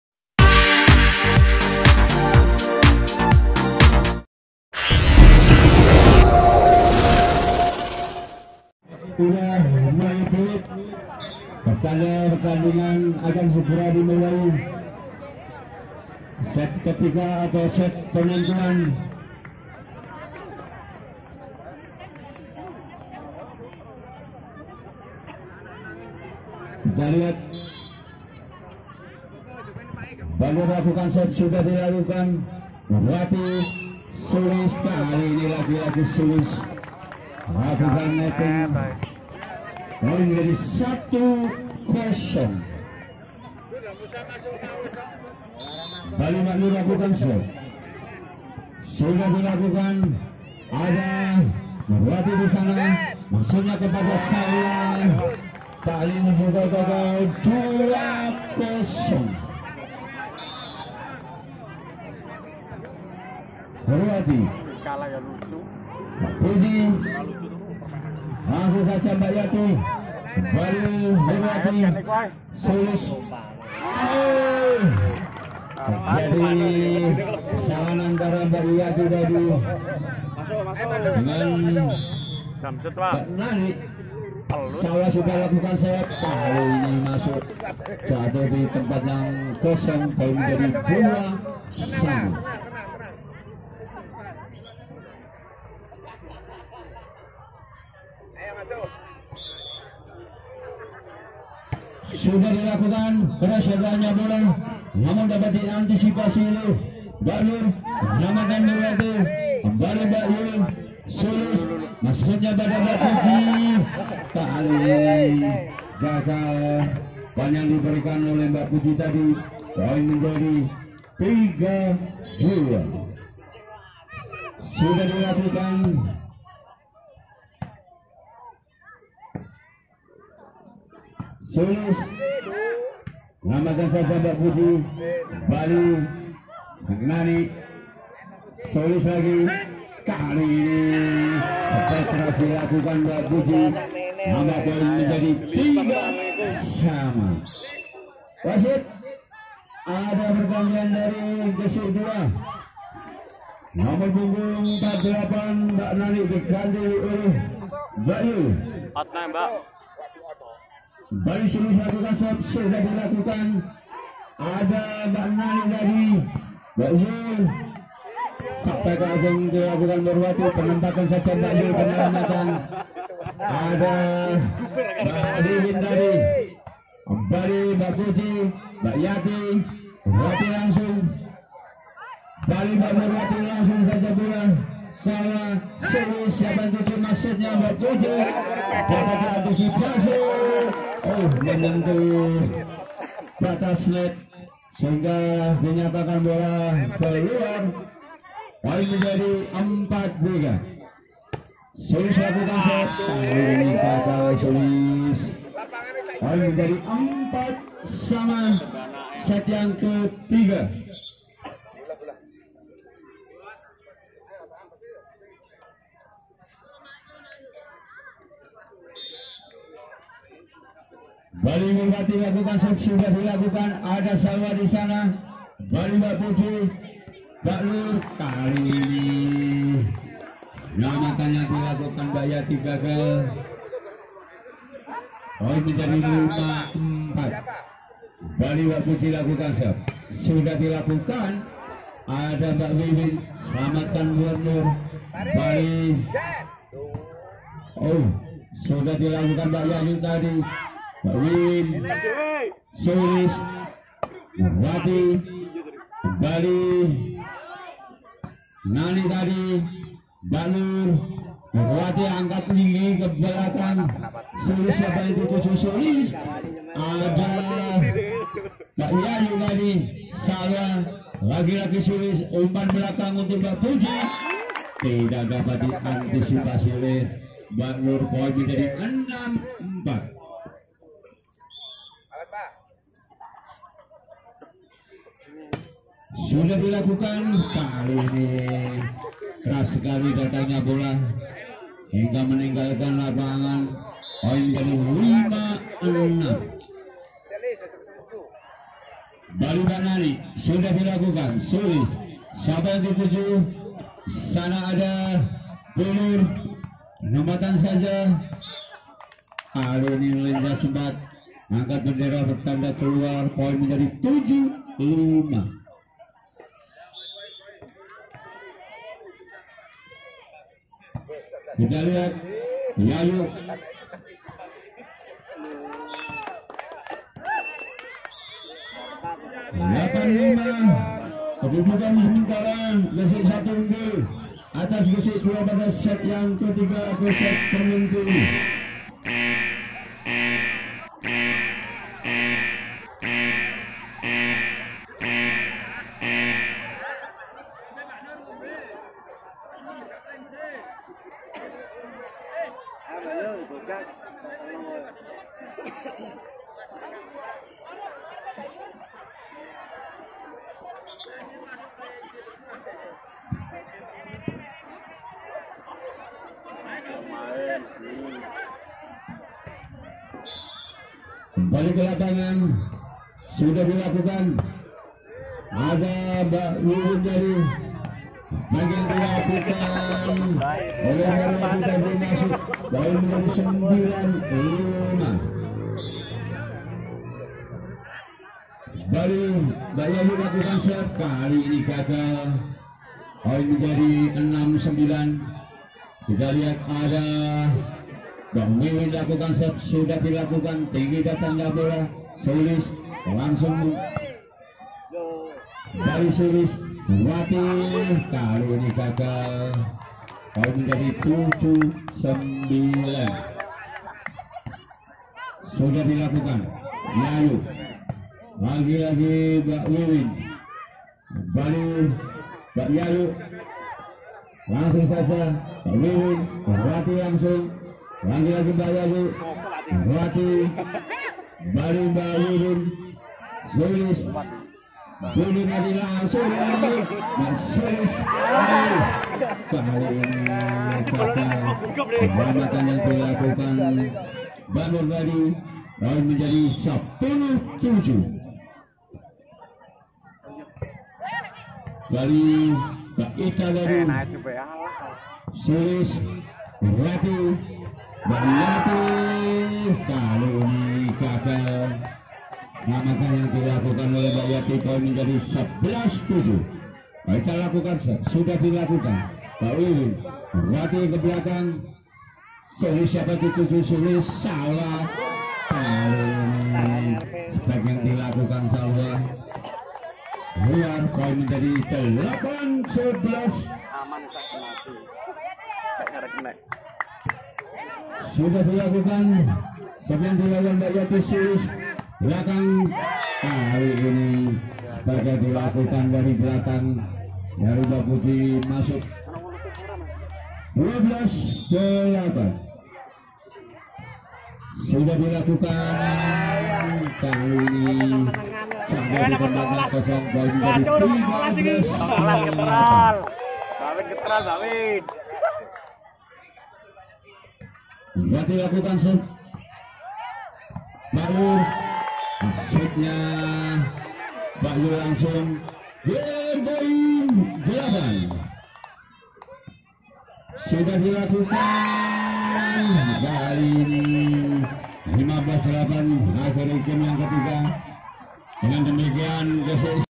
Turnamen Voli Putri‼️SET KE TIGA❕Gesik 1 vs Gesik 2...
Turnamen Desa Gesik harjo Antar RW Tahun 2021,,,
Melibatkan Dusun Gesik, Dusun Rembes Dan Dusun Gemulung...

Kegiatan Turnamen bola voli ini di agendakan akan di adakan satu tahun sekali,
agar bibit muda pemain voli bisa terus berkembang...